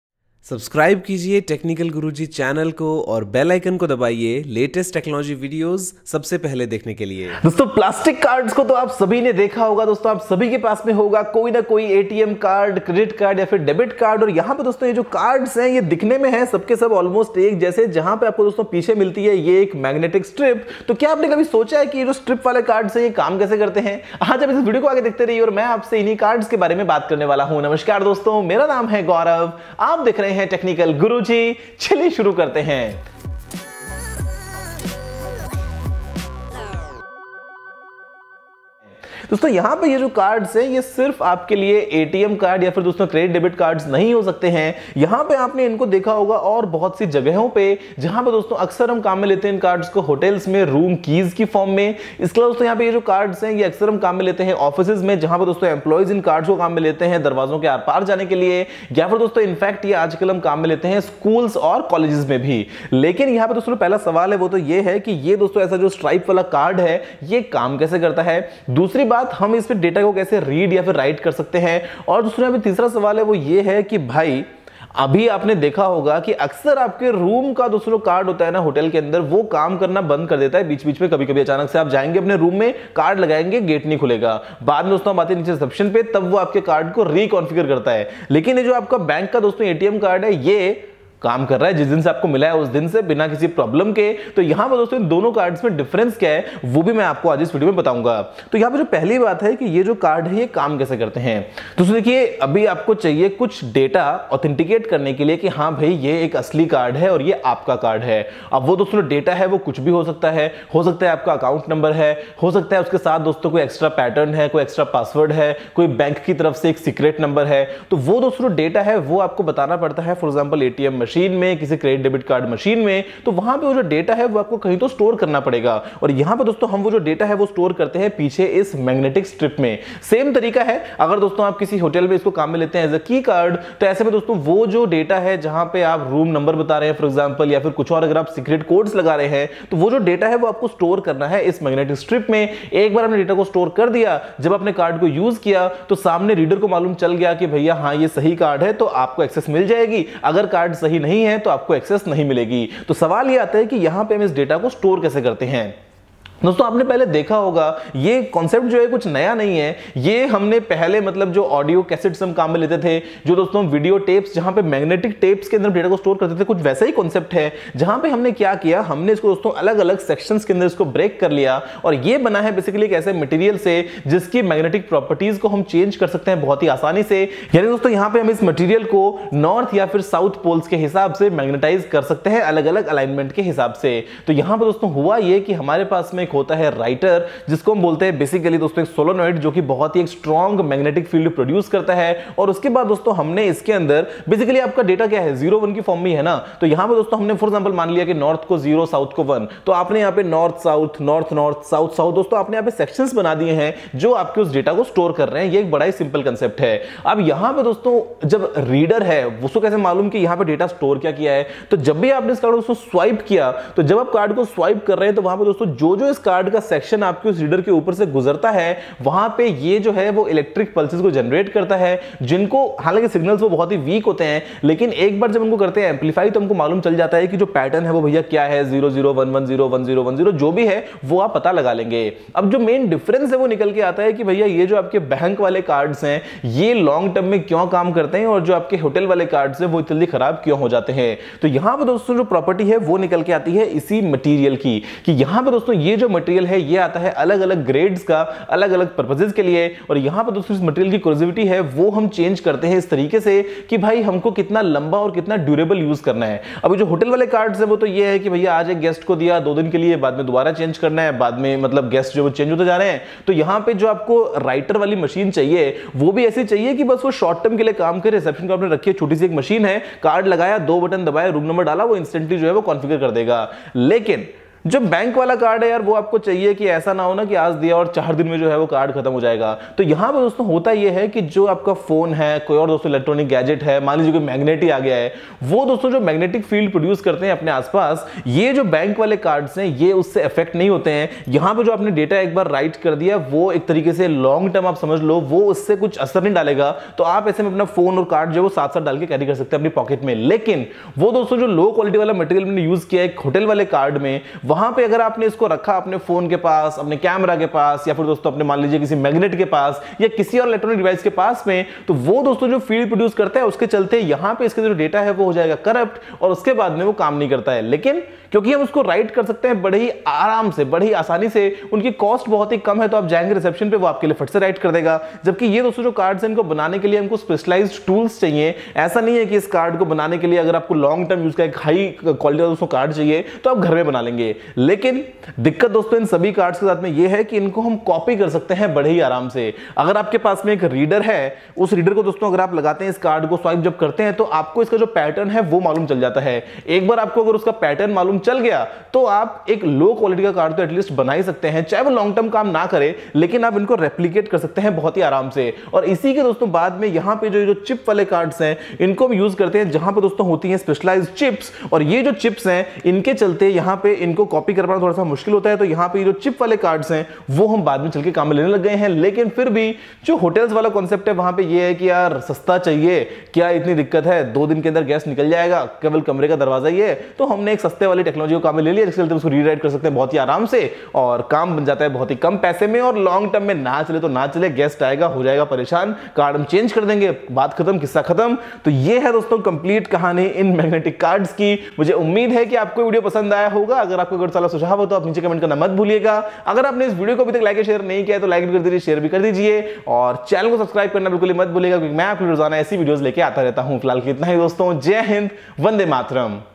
How Magnetic Stripe Cards Work? Hotel Key Cards Vs Bank Cards?
Namaskaar Dosto, is video mein maine aapse Magnetic Stripe Cars ke baare mein baat ki hai, yeh cards aapne dekhe honge ATM Cards, Credit Cards, Debit Cards ki form mein aur iske alawa Hotels mein Room Key Cards aur officies aur schools mein bhi hum in cards ko use karte hai. Yeh Mag Stripe cards kaise kaam karte hai? Kaise data inme store hota hai? Aur Hotels mein Cards aur Bank Cards mein kya difference hai wahi maine aapko is video mein bataya hai. Mujhe umeed hai ki aapko yeh video pasand aayega.